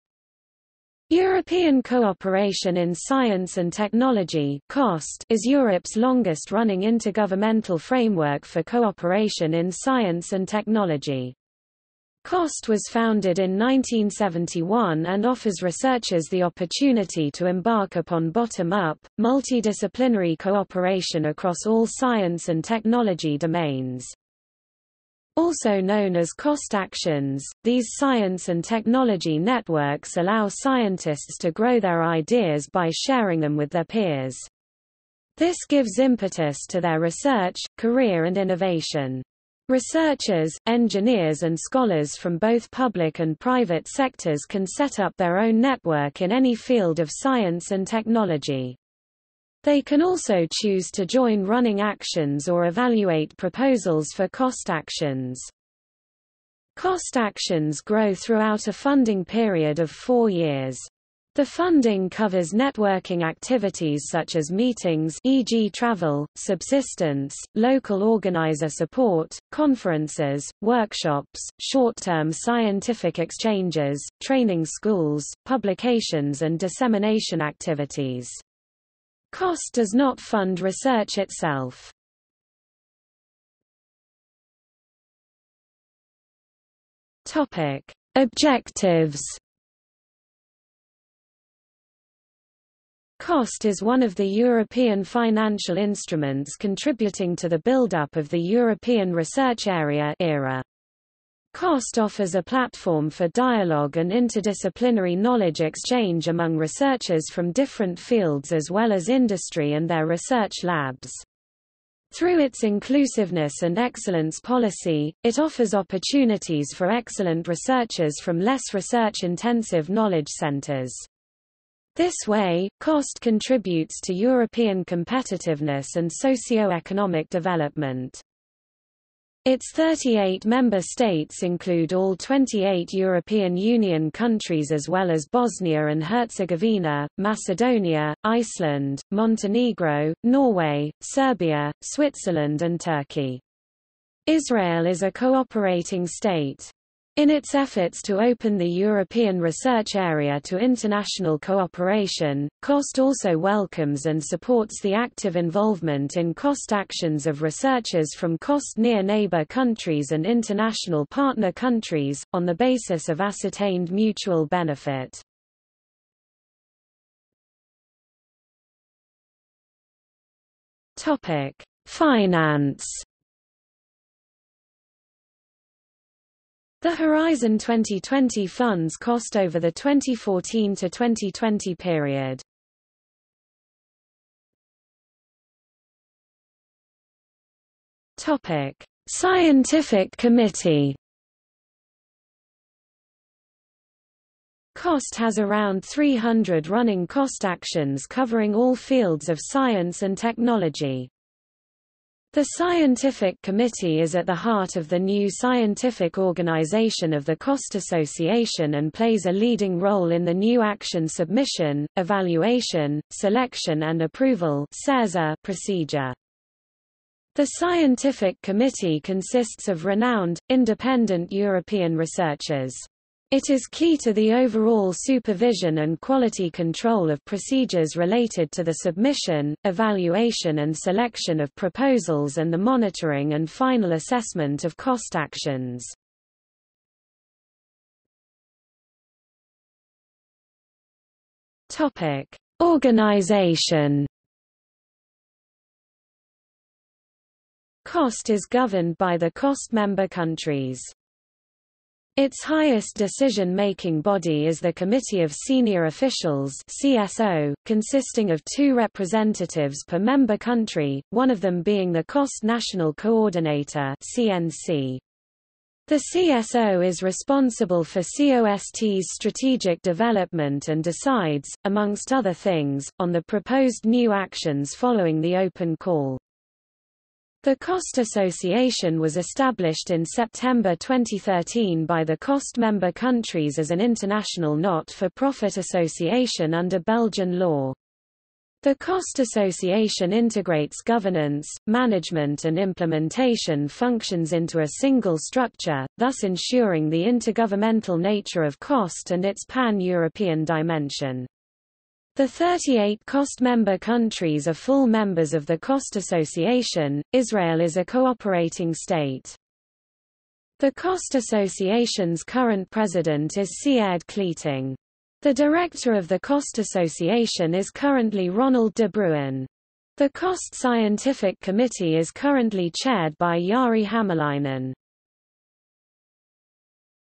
COST - European Cooperation in Science and Technology | Wikipedia audio article
00:01:23 1 Objectives
00:02:47 2 Finance
00:03:04 3 Scientific Committee
00:03:58 4 Organisation

- Socrates

SUMMARY
European Cooperation in Science and Technology (COST) is Europe's longest-running intergovernmental framework for cooperation in science and technology. COST was founded in 1971 and offers researchers the opportunity to embark upon bottom-up, multidisciplinary cooperation across all science and technology domains.
Also known as COST Actions, these science and technology networks allow scientists to grow their ideas by sharing them with their peers. This gives impetus to their research, career and innovation. Researchers, engineers and scholars from both public and private sectors can   set up their own network in any field of science and technology. They can also choose to join running Actions or evaluate proposals for COST Actions.
COST Actions grow throughout a funding period of 4 years. The funding covers networking activities such as meetings (e.g. travel, subsistence, local organiser support), conferences, workshops, short-term scientific exchanges, training schools, publications and dissemination activities. COST does not fund research itself.